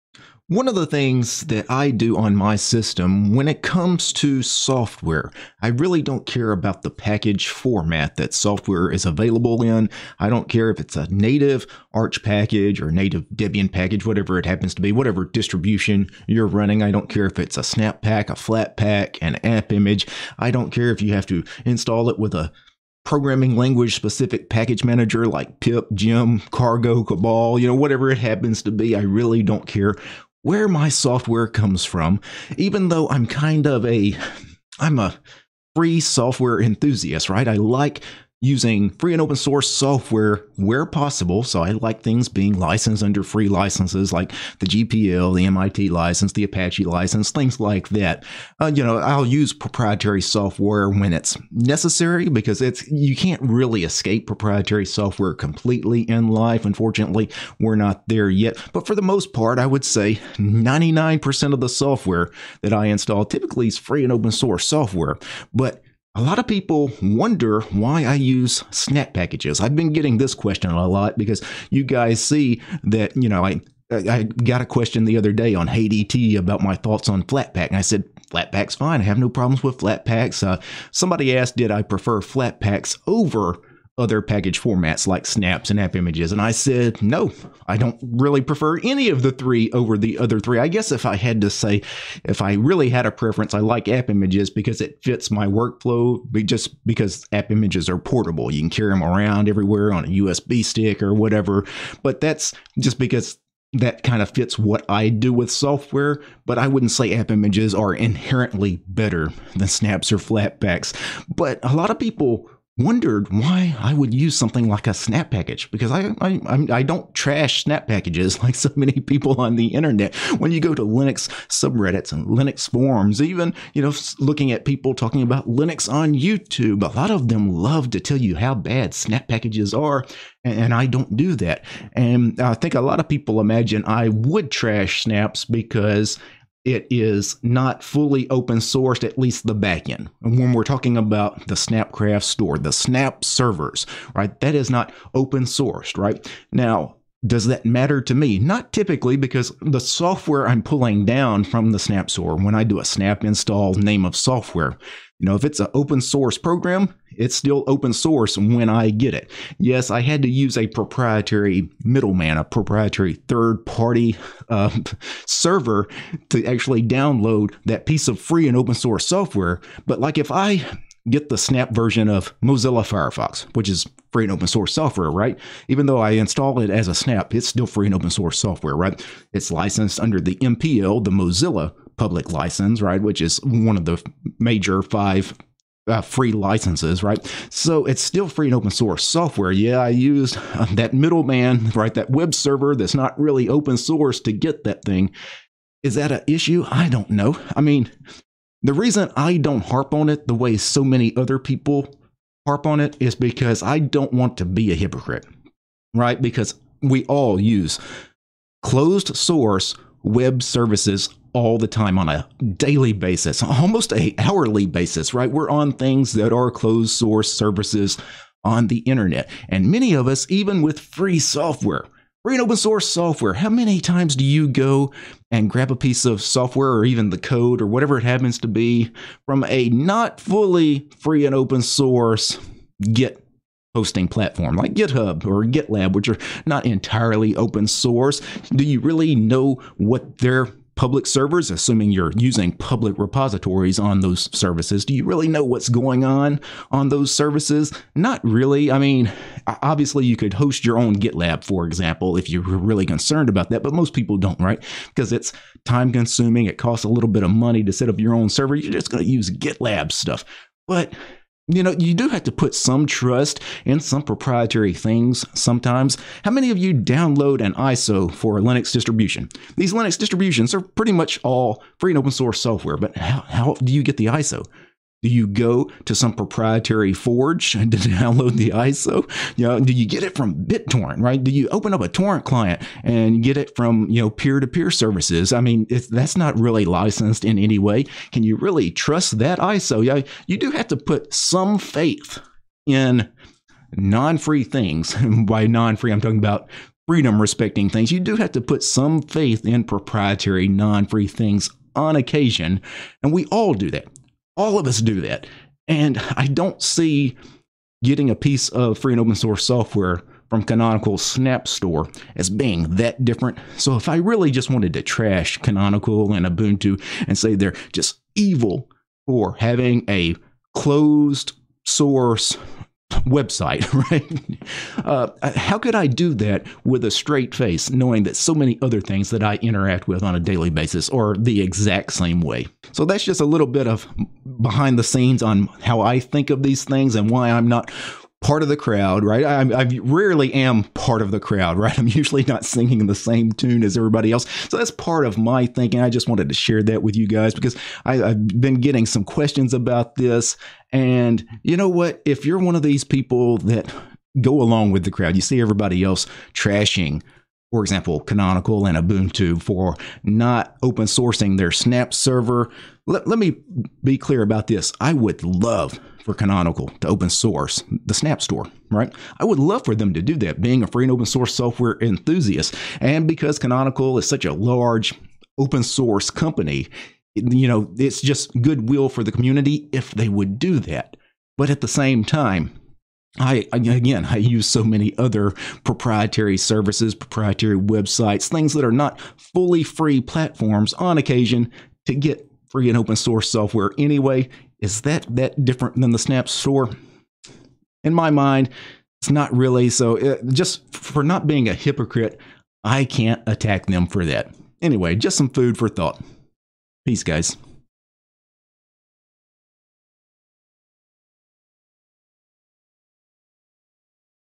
Is It WRONG To Use Snaps If You Love Open Source?
I've mentioned that recently I have installed several snap packages on my system due to some native Arch packages not working for me.  Naturally, some people want to why I, a free software advocate, use snap packages?